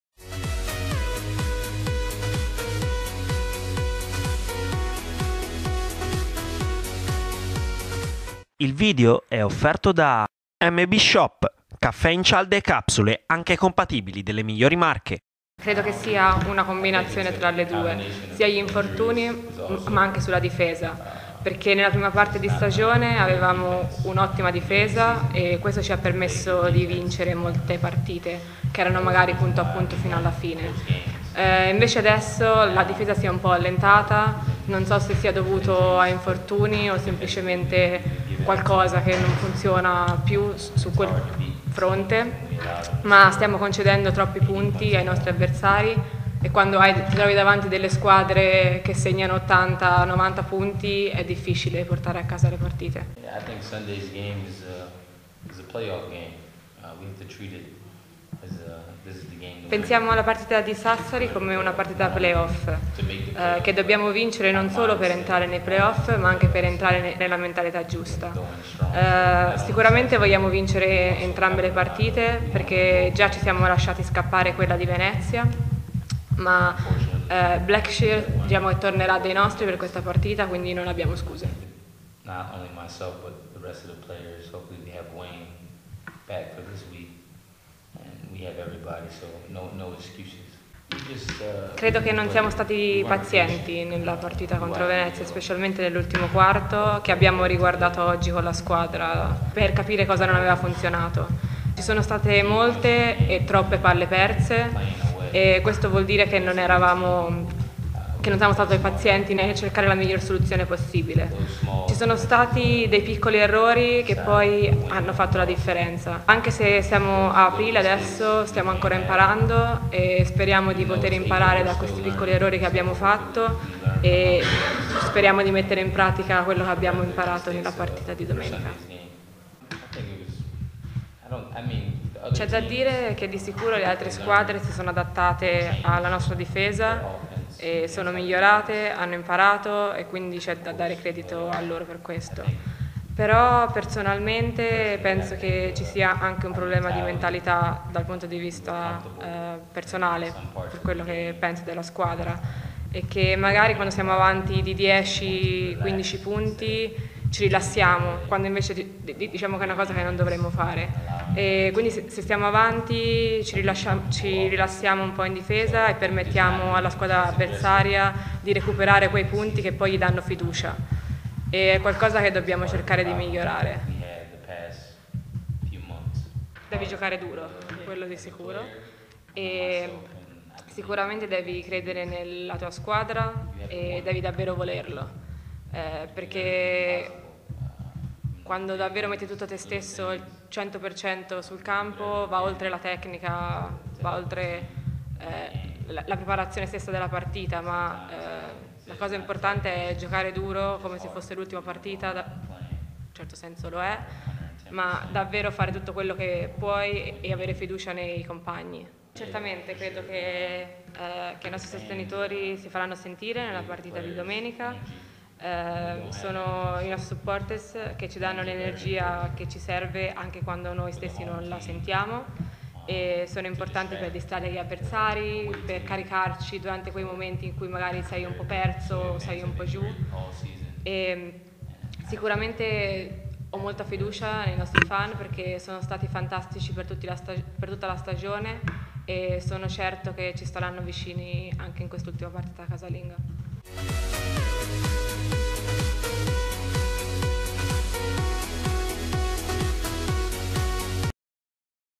Moore: "Domenica contro Sassari sarà come se fossimo già ai play off" [PistoiaBasketTV]
Pistoia Basket Tv - La web tv ufficiale della Giorgio Tesi Group - Pistoia Basket 2000